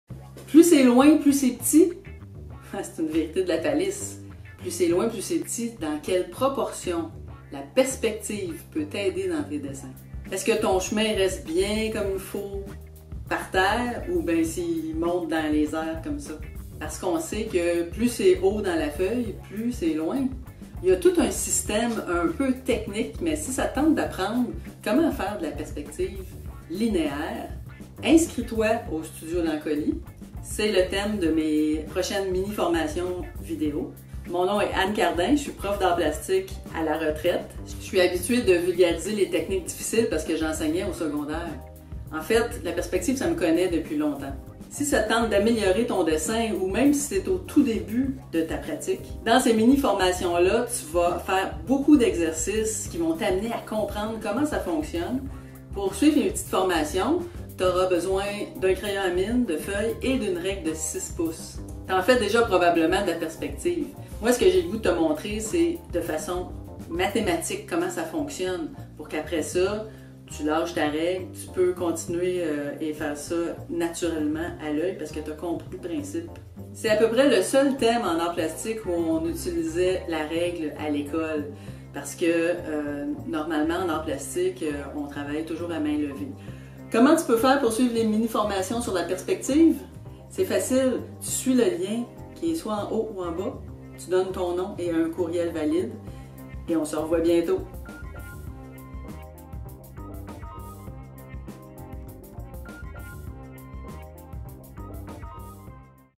La perspective en dessin
Plus c’est loin plus c’est petit? Mais encore! Réglez vos problèmes de perspective en suivant mes mini-formations en dessin. Inscrivez-vous au Studio L’Ancolie, c’est gratuit!